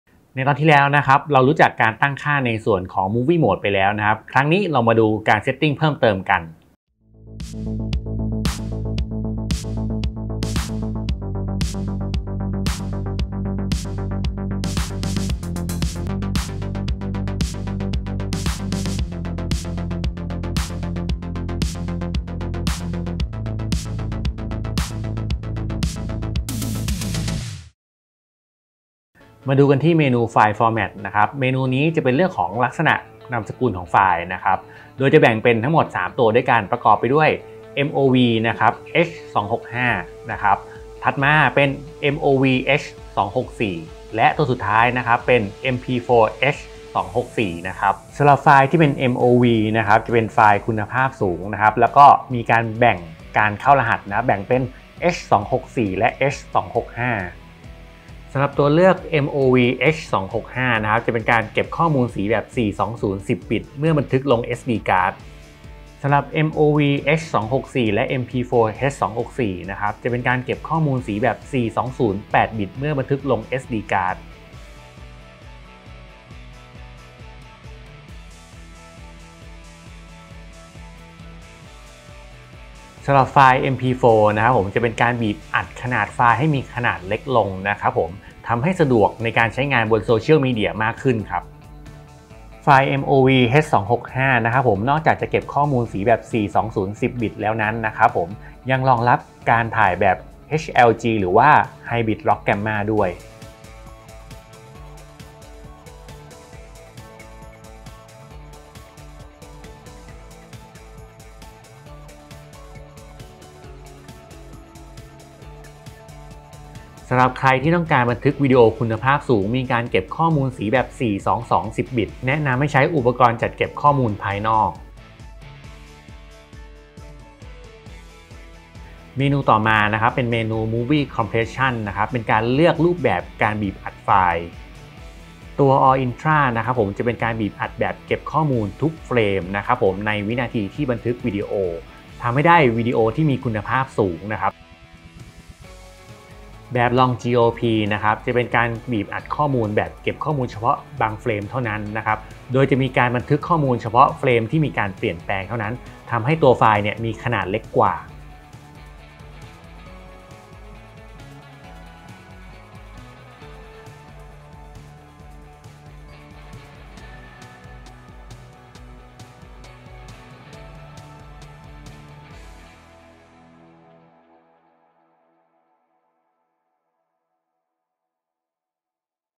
X-Tutorial : X-T4 EP.9
X-T4 Tutorial EP.9 กับ Basic Movie Setting
ในหัวข้อของ File Format และ Movie Compression

สำหรับใครที่สนใจอยากมา Touch & Try กล้อง X-T4
ที่ FUJIFILM X-Space สามารถอ่านรายละเอียดเพิ่มเติม